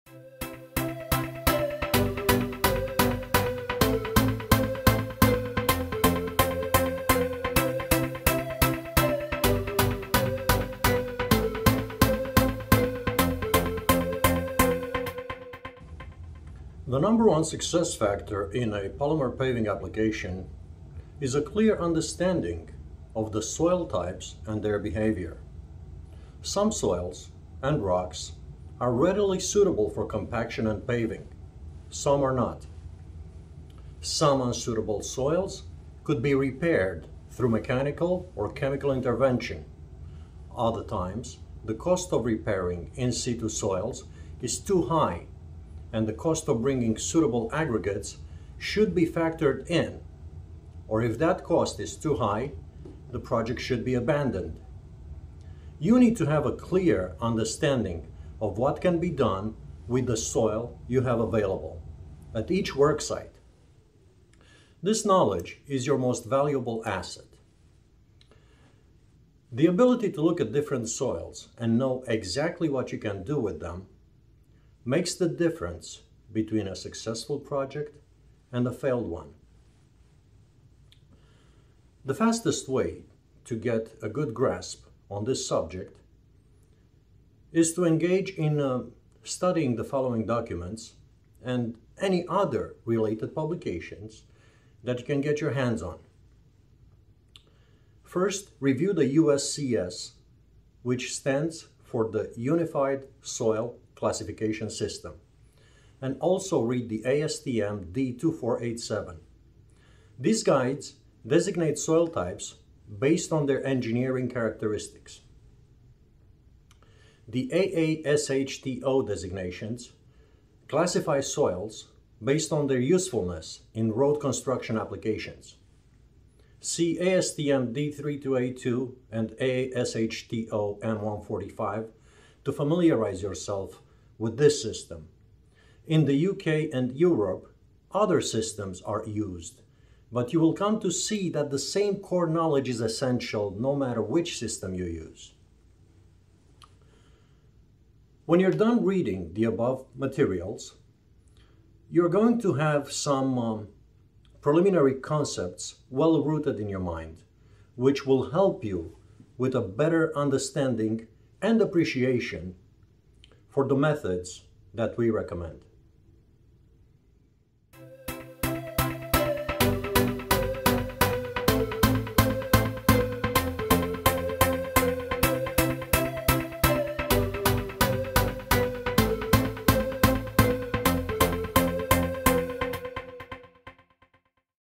Preliminary considerations - polymer paving
This short video shares the reading materials necessary for understanding the core geotechnical characteristics of common soil types.